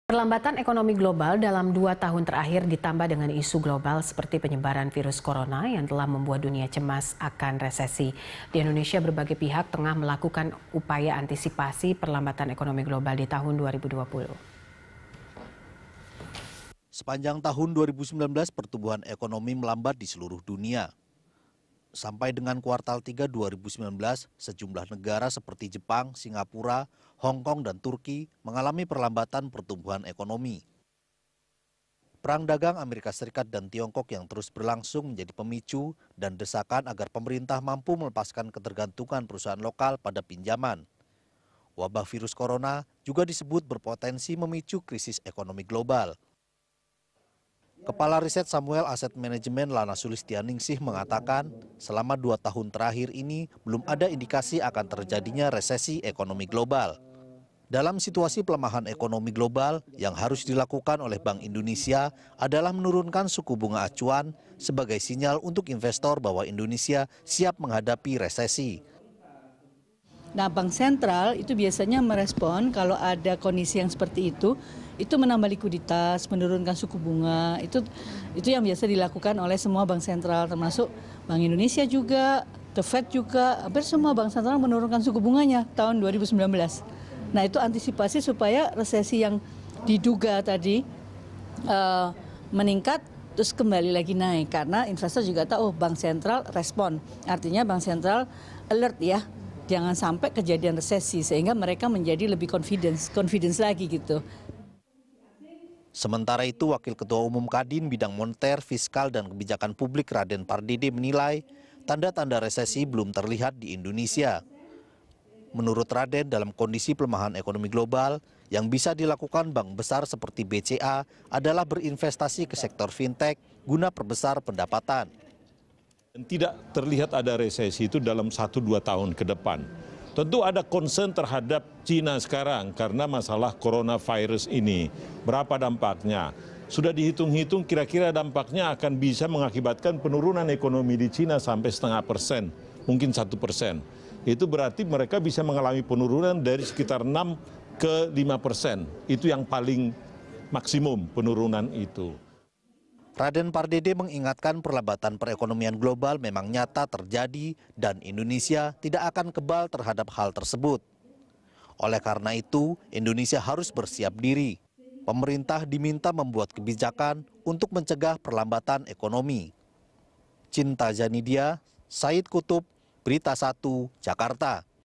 Indonesia Antisipasi Perlambatan Ekonomi Global
Perlambatan ekonomi global dalam dua tahun terakhir ditambah isu global seperti penyebaran virus corona membuat dunia cemas akan resesi. Di Indonesia, berbagai pihak tengah berupaya mengantisipasi perlambatan ekonomi global di tahun 2020.

Facebook.com/BeritaSatu
Youtube.com/BeritaSatu